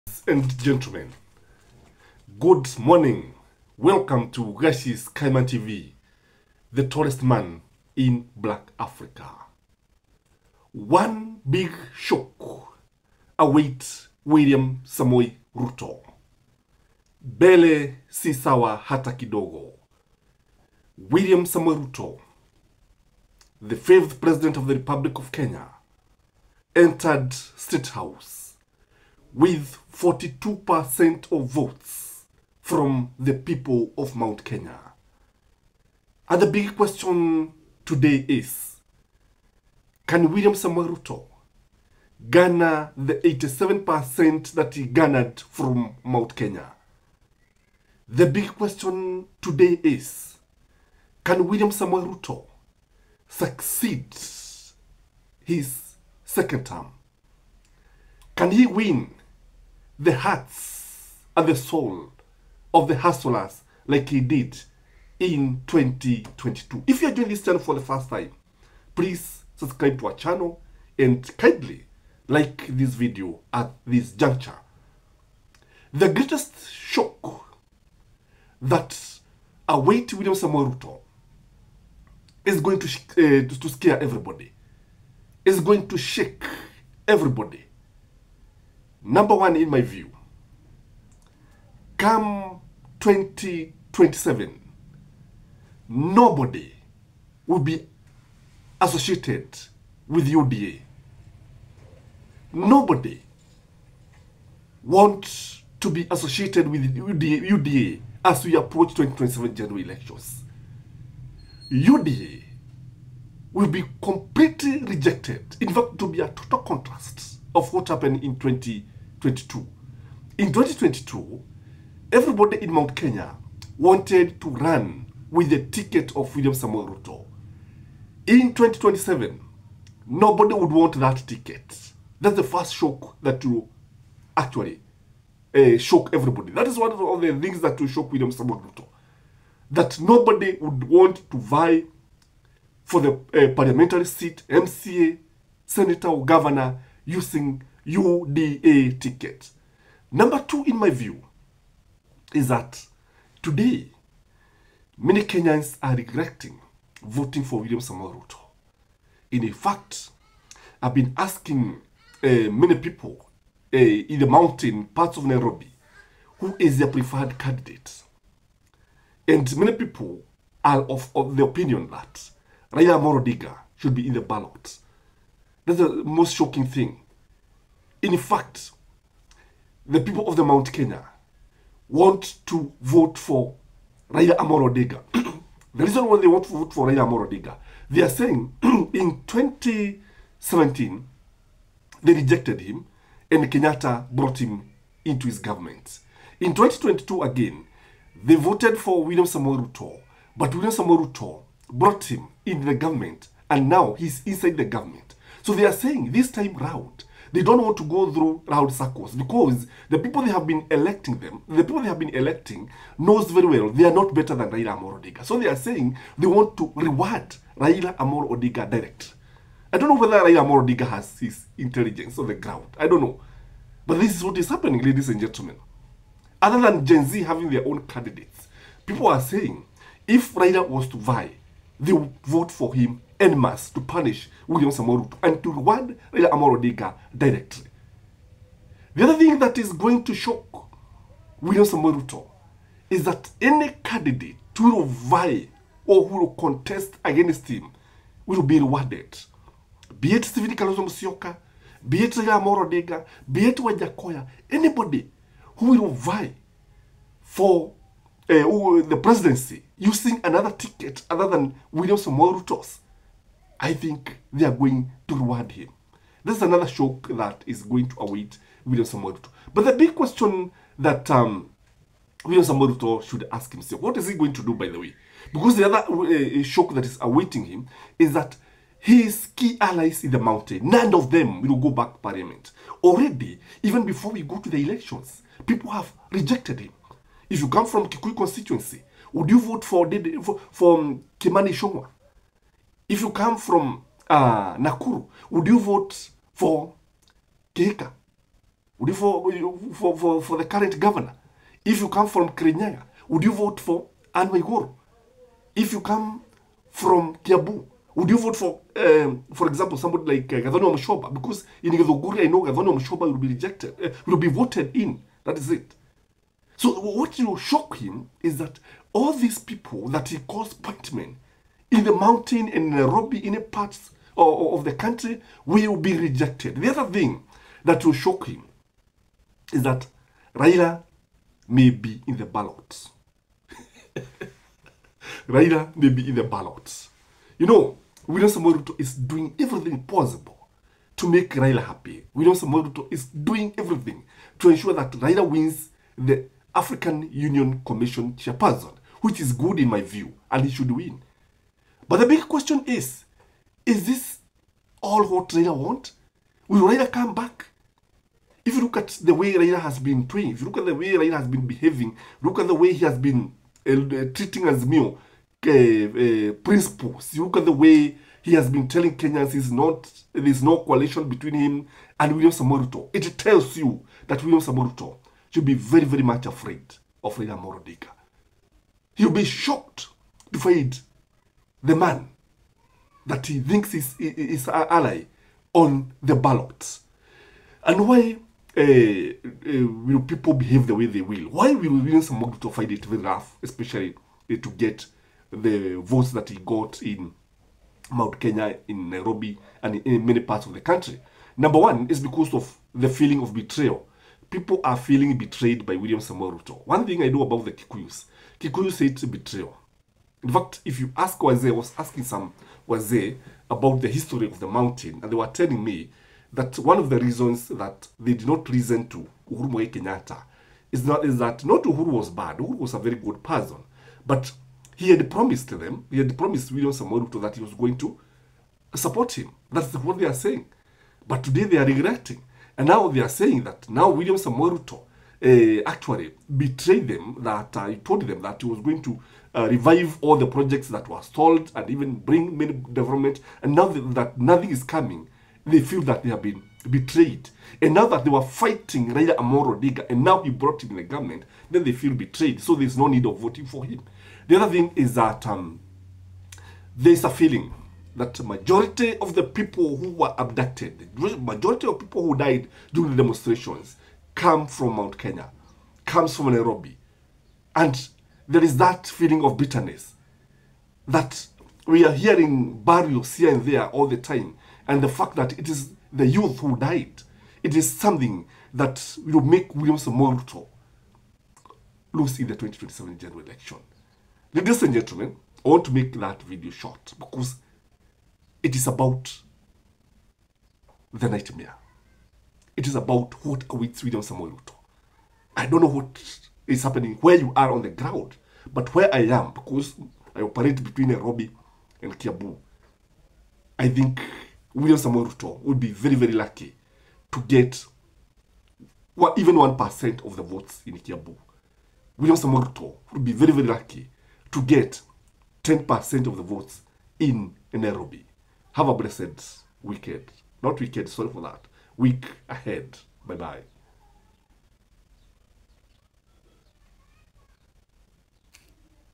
RUTO'S MAGIC CANDLE SET ABLAZE
The growing discontent against Kenya Kwanza administration  the unkept promises and the high cost of living is turning into a nightmare for Ruto re election bid.in this video we give a pre elections presidential analysis of 2027 and how the tables are likely yo turn,the surprises   and the unexpected outlook. Gachie Skyman TV is an international news channel focusing on breaking news  current  ,educational and entertaining news.